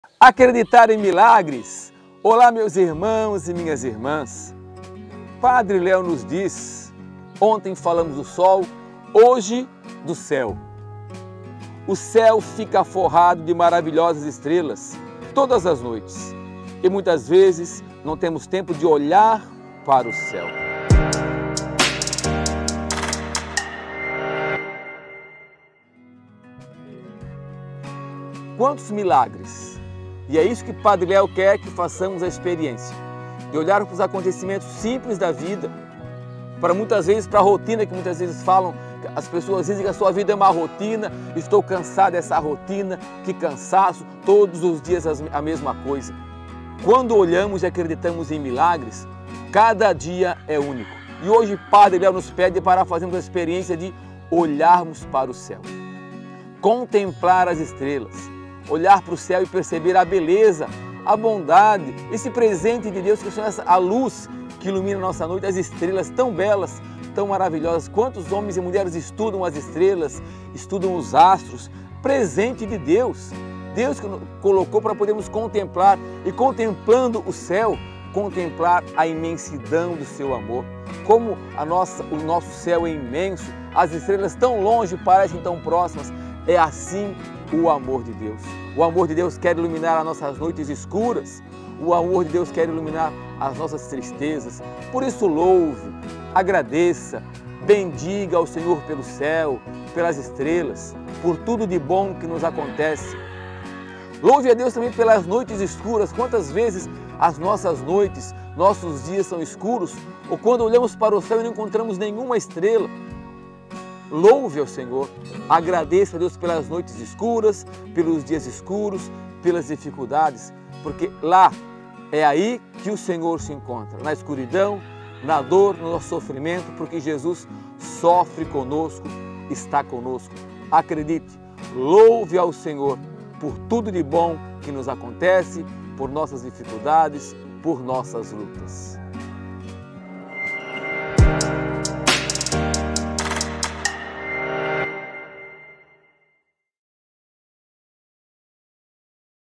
Louvar a Deus por nossas lutas
Gotas de Cura Interior - 124 -

Pela graça da cura interior podemos chegar a oração do coração

Com diácono Ideraldo Paloschi, cofundador da Comunidade Bethânia, fundada pelo Servo de Deus Padre Léo

Comunidade Bethânia - Pe. LÉO Oficial